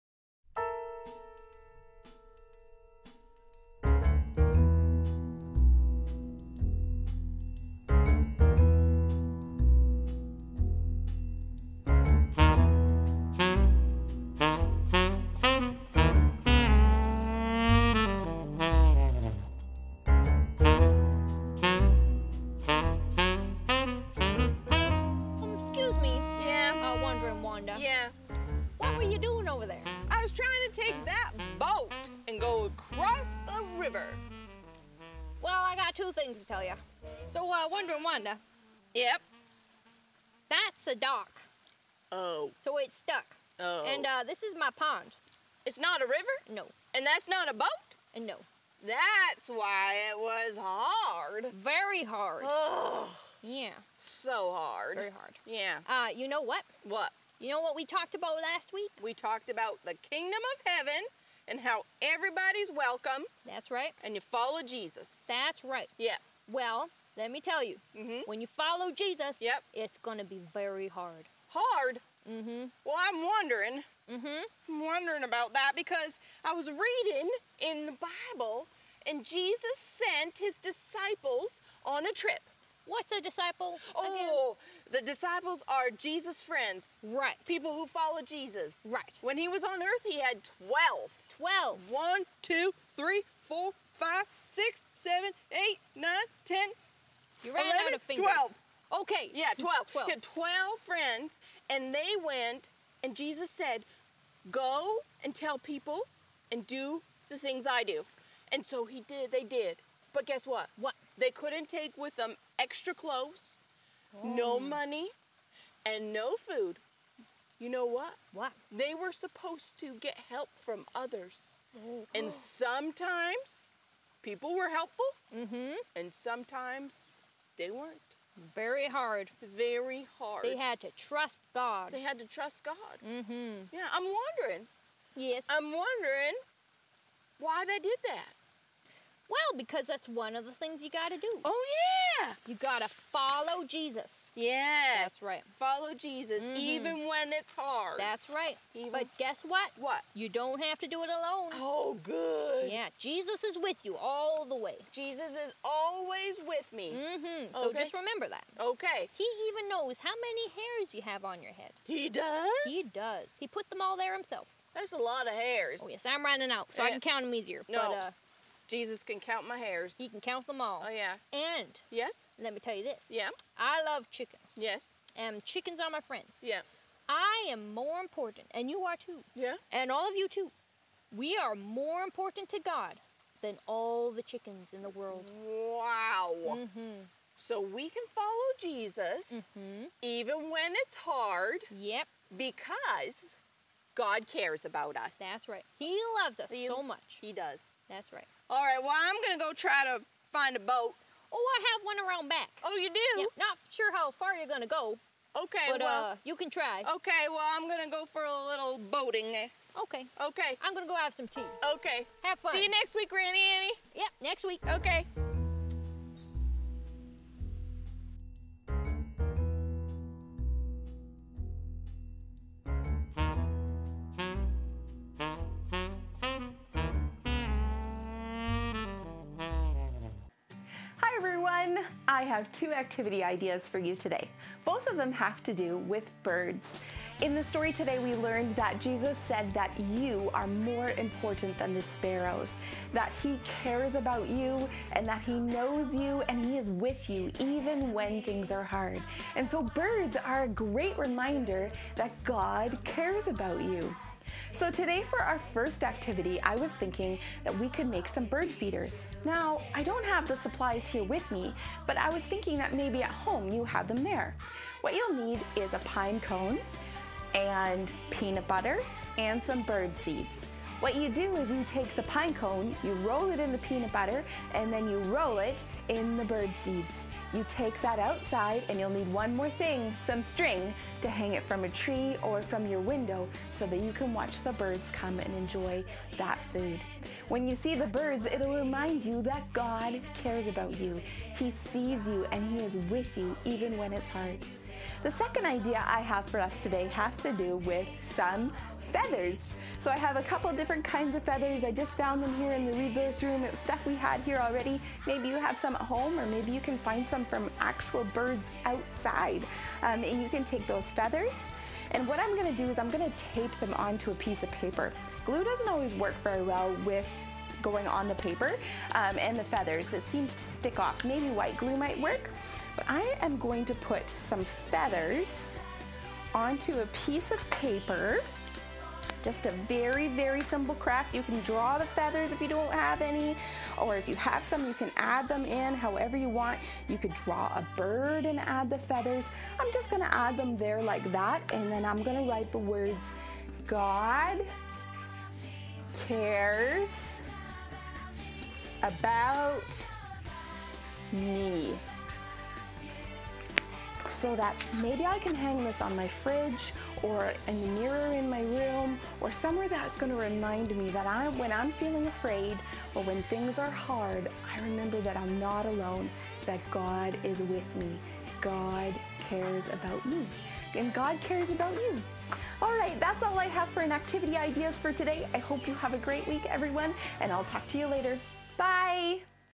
Rooted Kids - Preschool - Wanted Series - Lesson 2
God cares about you!  He is with you all the time. He sees you.  (Matthew 10)